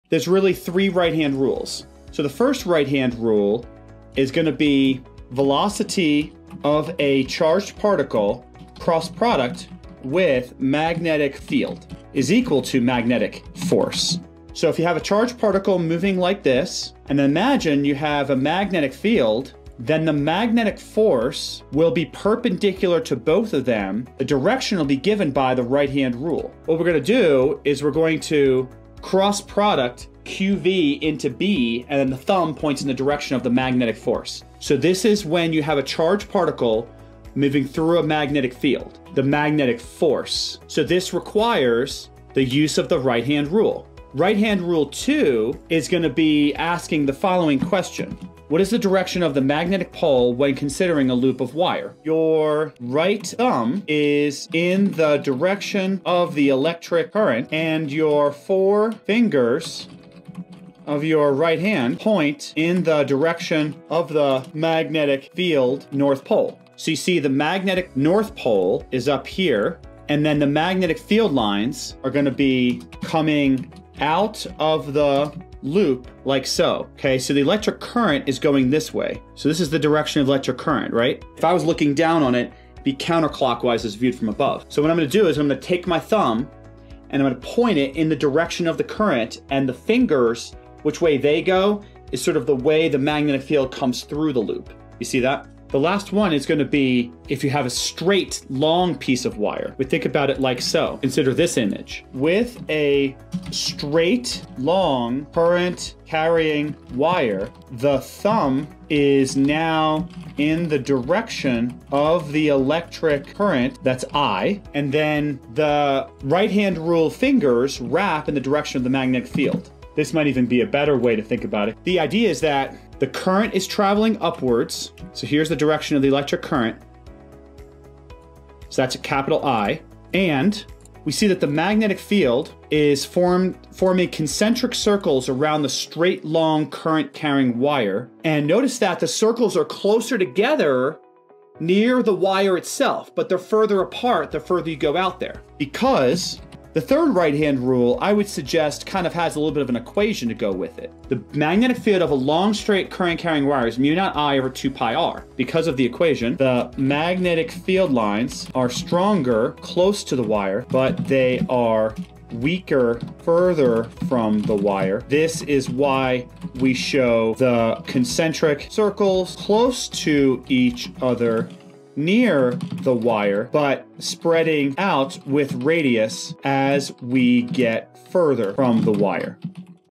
Right-Hand Tutorial | Right-Hand Rule Physics 2
In this video, we explain the three right-hand rules used in physics and electromagnetism. These rules help you quickly determine the direction of magnetic force, magnetic fields, and current flow — essential for students preparing for exams and anyone learning electromagnetism.